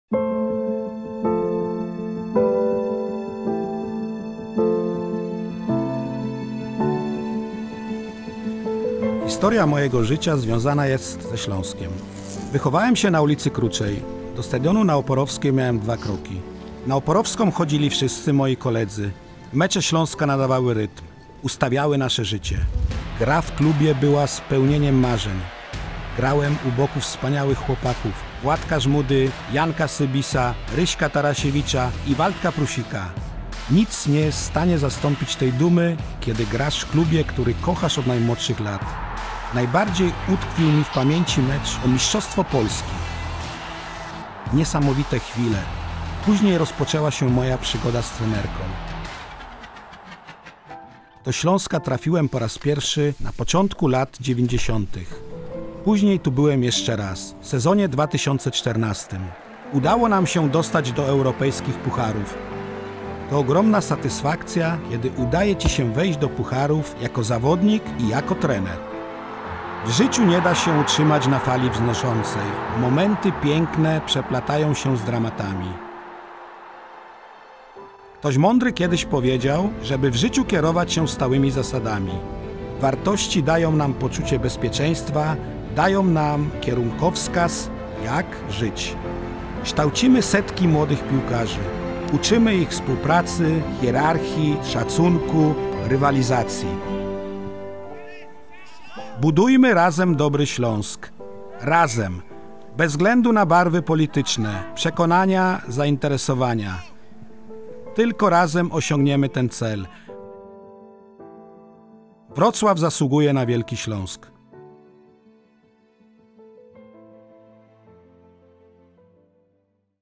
Śląsk. Wrocław.
"Wrocław znaczy Śląsk. Wspólnie budujmy Klub, z którego wszyscy możemy być dumni".

Od podawania piłek na stadionie przy Oporowskiej, po występy w I zespole, gol z Liverpoolem, mistrzostwo Polski, powrót w roli trenera, aż po wychowywanie kolejnych młodych graczy w akademii piłkarskiej. Jedno życie. Jeden Klub. Tadeusz Pawłowski.